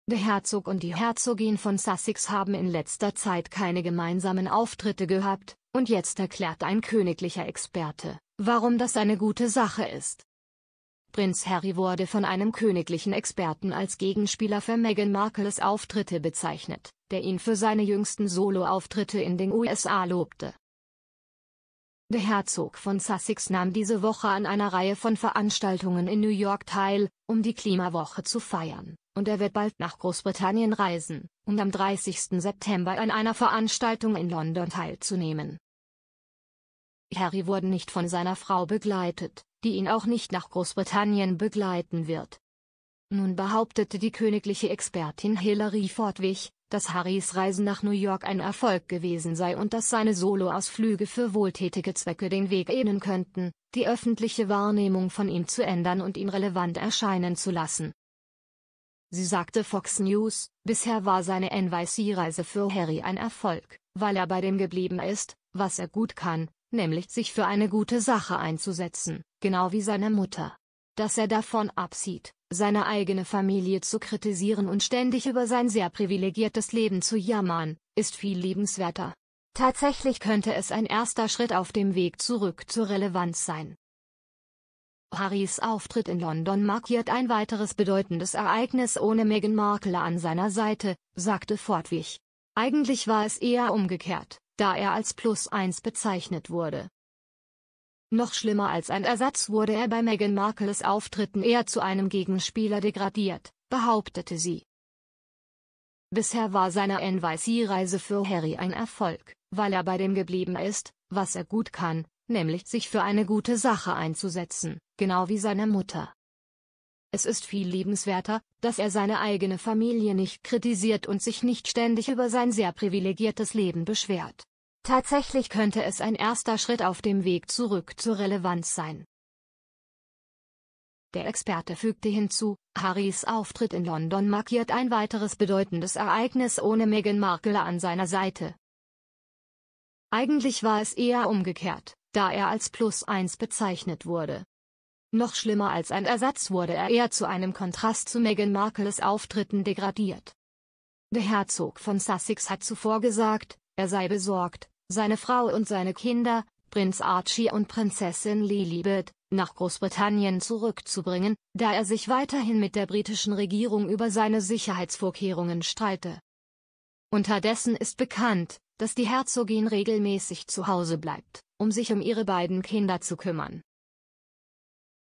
Prinz Harry schüttelt das Label „Plus Eins“ zu Meghan Markle ab, während er endlich solo geht – Exp
Der Herzog und die Herzogin von Sussex haben in letzter Zeit keine gemeinsamen Auftritte absolviert und nun erklärt ein königlicher Experte, warum das eine gute Sache ist.